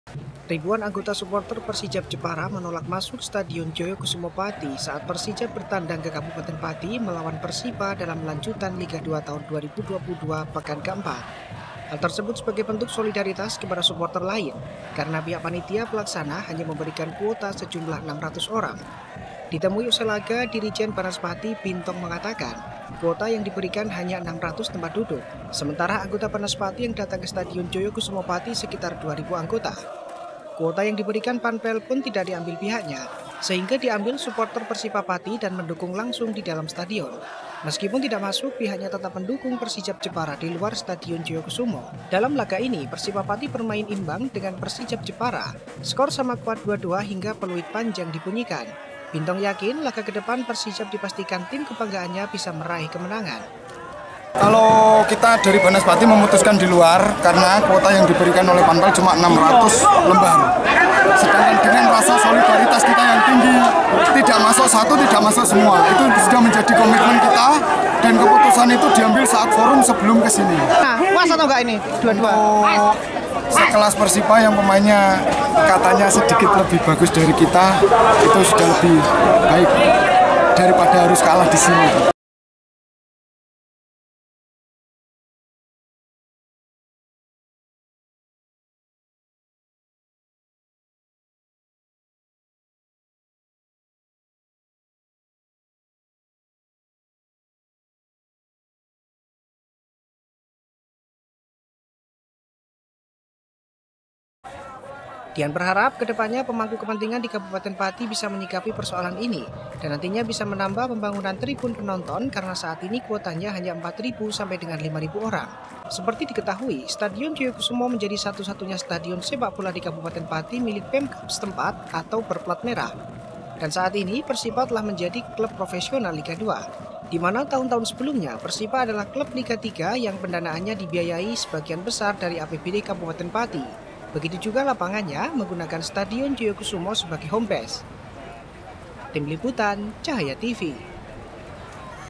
Siasati kuota terbatas, ribuan suporter persijap dukung tim nya dari luar stadion joyokusumo
Ribuan anggota suporter persijap jepara, menolak masuk stadion joyokusumo pati saat persijap bertandang ke kabupaten pati melawan persipa dalam lanjutan liga dua tahun 2022 pekan ke empat. hal tersebut sebagai bentuk solidaritas kepada suporter lain,karena pihak panitia pelaksana hanya memberikan kuota sejumlah 600 orang..